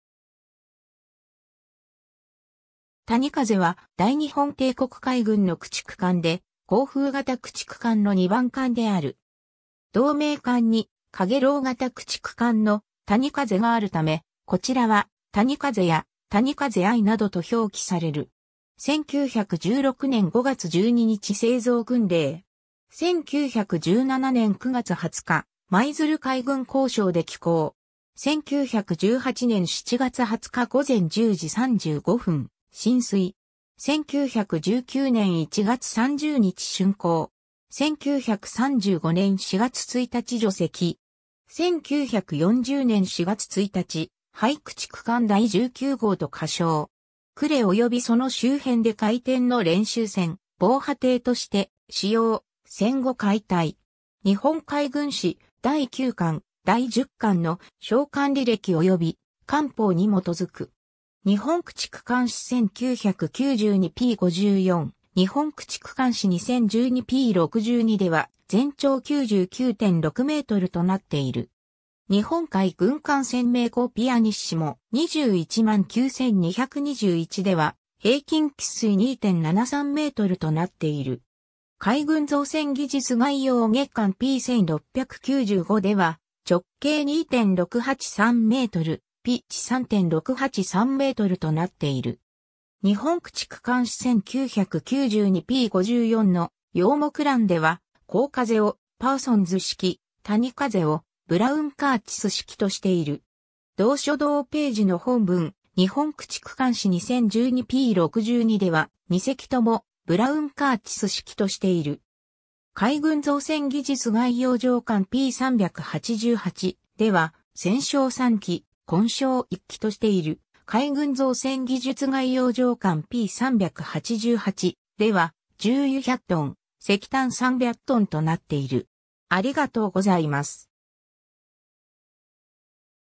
谷風 (江風型駆逐艦)
谷風（たにかぜ）は、大日本帝国海軍の駆逐艦で、江風型駆逐艦の2番艦である。
同名艦に陽炎型駆逐艦の「谷風」があるため、こちらは「谷風 (初代)」や「谷風I」などと表記される。
 1916年（大正5年）5月12日製造訓令。
1917年（大正6年）9月20日、舞鶴海軍工廠で起工。
1918年（大正7年）7月20日午前10時35分進水。
1919年（大正8年）1月30日竣工。
 1935年（昭和10年）4月1日除籍。
1940年（昭和15年）4月1日、廃駆逐艦第19号と仮称。
呉及びその周辺で回天の練習船、防波堤として使用、戦後解体。
^ 日本駆逐艦史1992p.54、日本駆逐艦史2012p.62では、全長99.6mとなっている。
^ 日本海軍艦船名考pp.219,221では平均吃水2.73mとなっている。
^ 海軍造船技術概要(1987)下巻p.1695では、直径2.683m、ピッチ3.683mとなっている。
^ 日本駆逐艦史1992p.54の要目欄では、江風をパーソンズ式、谷風をブラウン・カーチス式としている。
同書同頁の本文、日本駆逐艦史2012p.62では、2隻ともブラウン・カーチス式としている。
^ 海軍造船技術概要(1987)上巻p.388では専焼3基、混焼1基としている ^ 海軍造船技術概要(1987)上巻p.388では重油100トン、石炭300トンとなっている